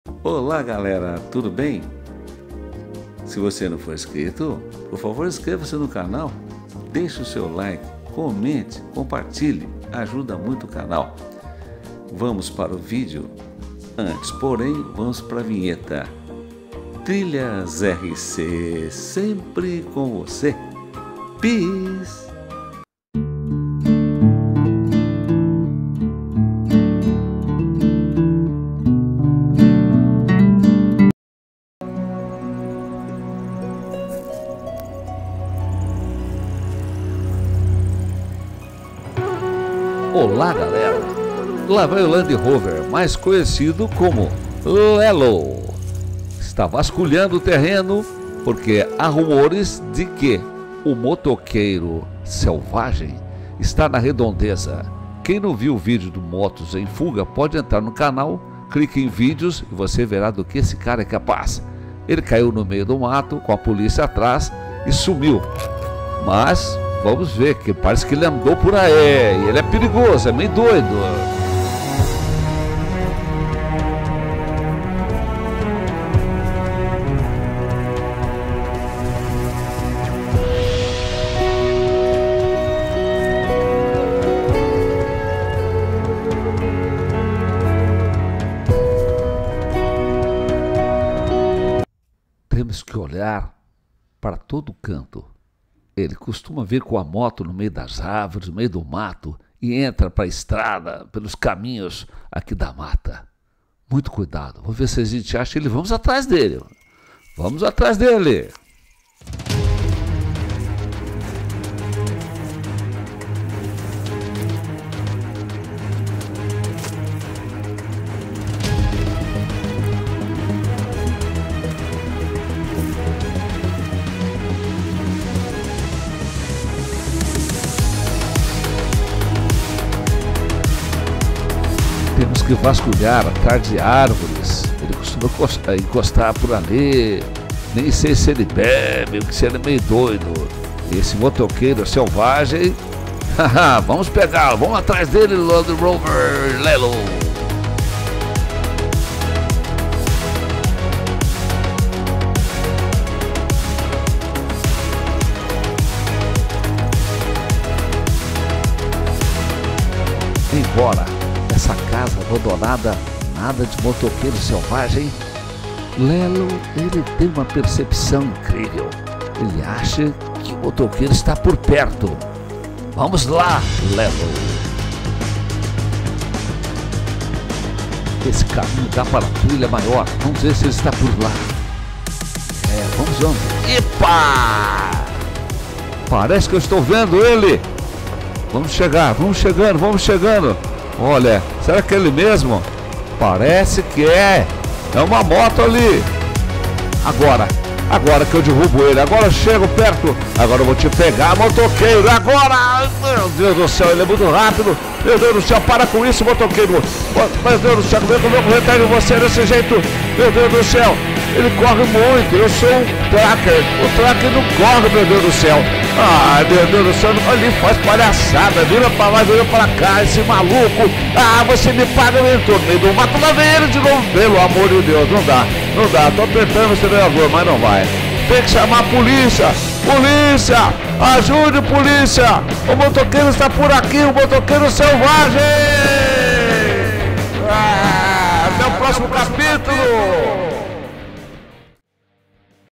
MOTOQUEIRO SELVAGEM
NOSSO LELLOW(LAND ROVER), FOI VASCULHAR A MATA ATRÁS DO MOTOQUEIRO SELVAGEM,PÓS RUMORES NA REDONDEZA,QUE ELE ESTARIA RONDANDO A ÁREA.